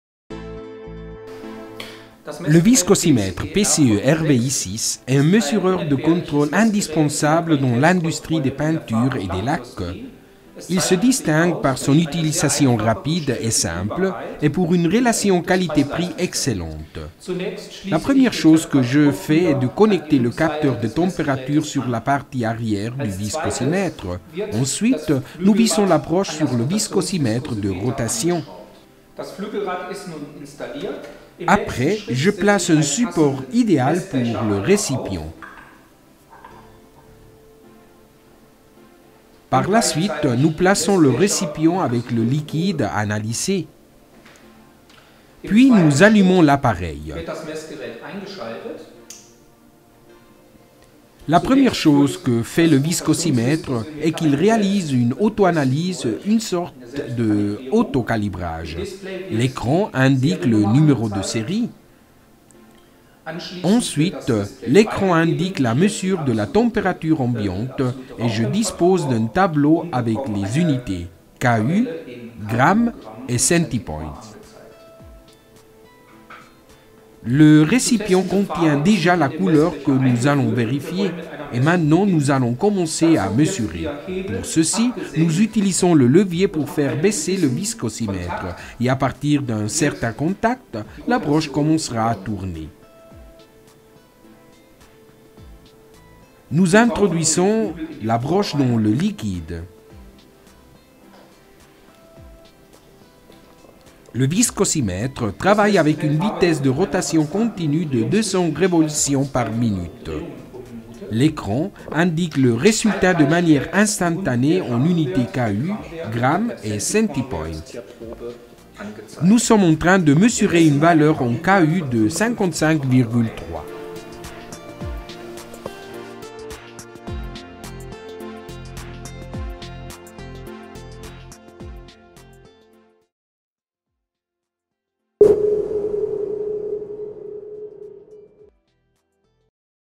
Notice emploi del viscosimètre PCE-RVI 6
Volker Berndt décrit le fonctionnement du viscosimètre PCE-RVI 6
Spécifications techniques PCE-RVI 6
Plage de mesure: 40,2 KU ... 141,0 KU / 32 g ... 1099 g / 27 cP ... 5250 cP (1cP = 1 mPa*s)
Déviation de la mesure: ±1,5 % du fond d'échelle
Reproductibilité: ±1,0 % du fond d'échelle
Vitesse de l'hélice: 200 r.p.m. ±0,5
Volume de l'éprouvette: environ 600 ml
Tension d'entrée: 220 V, 50 Hz
Pour plus d’informations sur PCE Instruments: